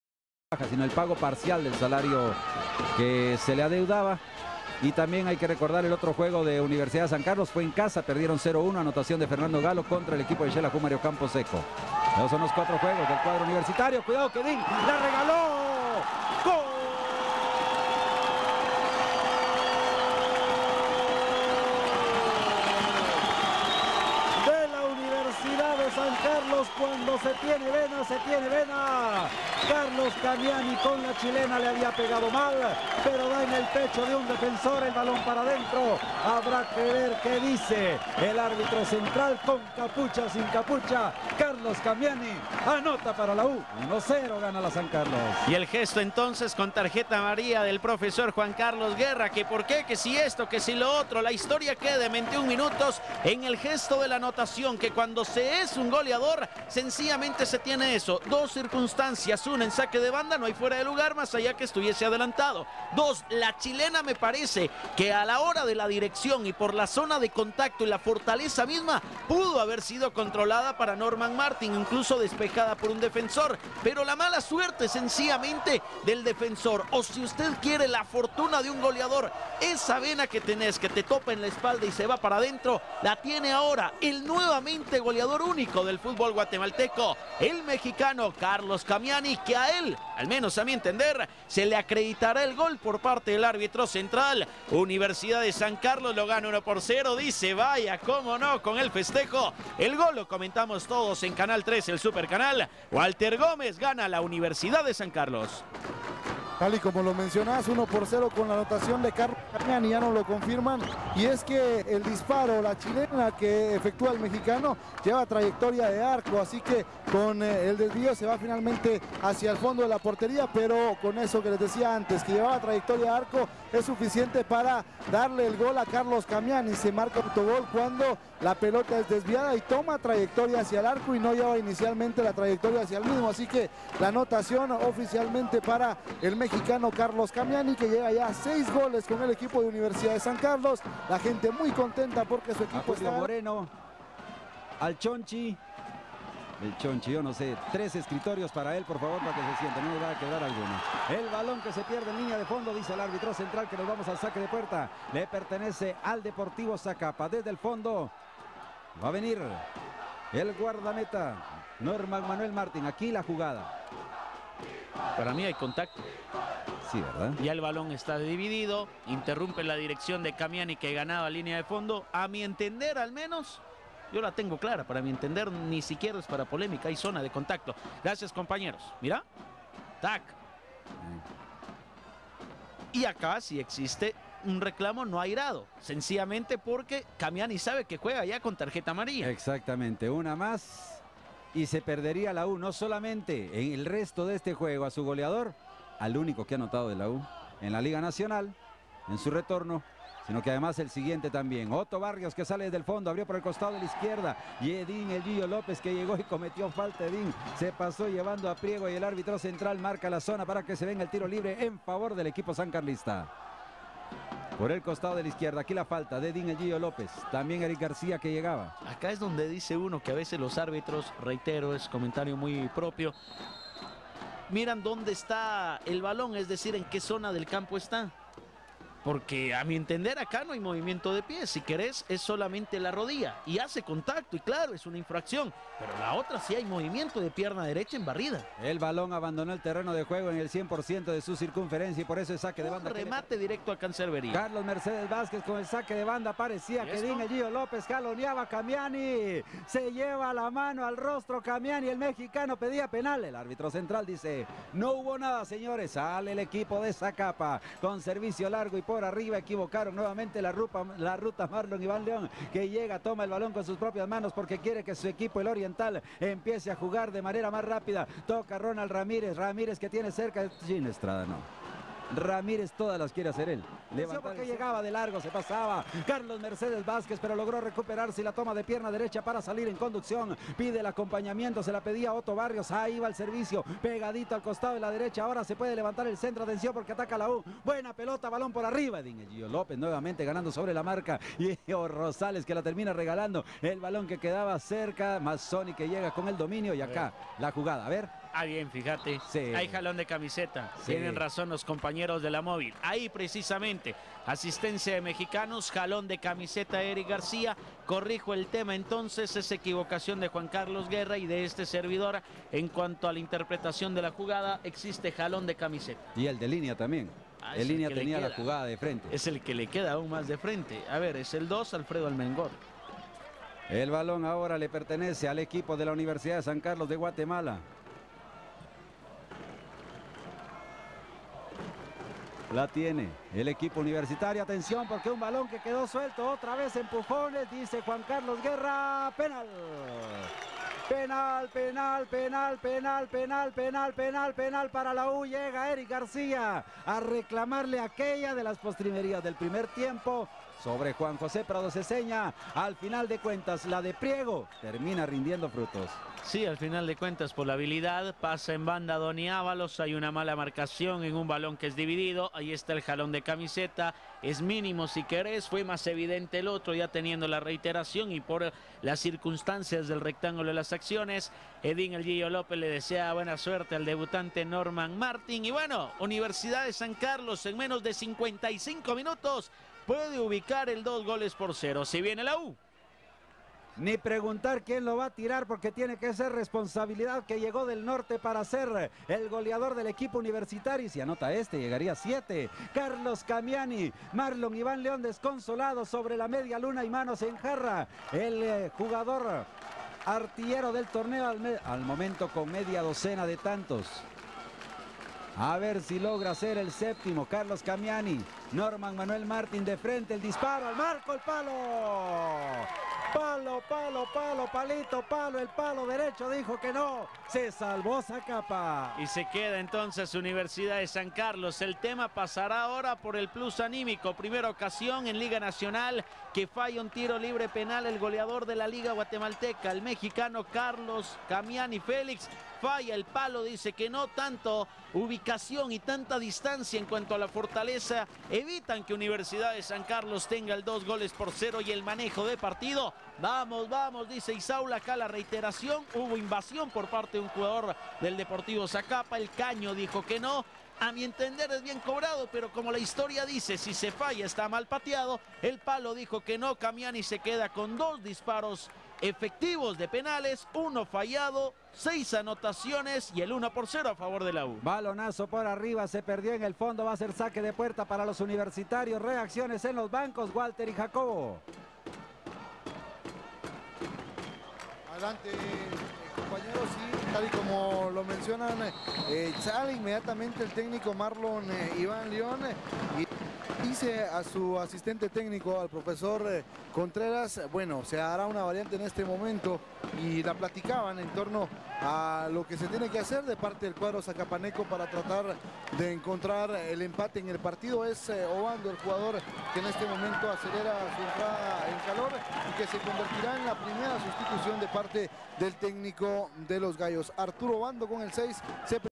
USAC vs Zacapa (22 de Agosto de 2009) EN VIVO
Resumen del partido correspondiente a la fecha número 5 del torneo de apertura de la liga mayor del futból de Guatemala transmitido por canal 3, el cual se disputó en el Mítico "Estado Revolución", la casa del equipo de la Universidad de San Carlos de Guatemala (USAC).

El marcador final fue: USAC 1 - Zacapa 1

Goles: Carlos Kamiani (USAC, minuto 21, chilena), Erick Priego (Zacapa, minuto 91, penal)
Arbitro: Juan Carlos Guerra

Curiosidades:

1. Carlos Kamiani fue amonestado con tarjeta amarilla al minuto 21 cuando celebraba el gol porque se puso una capucha en la cabeza.
2. Erick Priego cuando celebraba su gol paso frente a las 2 porras de la USAC provocandolos y no fue amonestado. Al parecer el arbitro desconoce que la FIFA exige que todo jugador que provoca al público debe ser amonestado.

El blog oficial del equipo de futbol de la USAC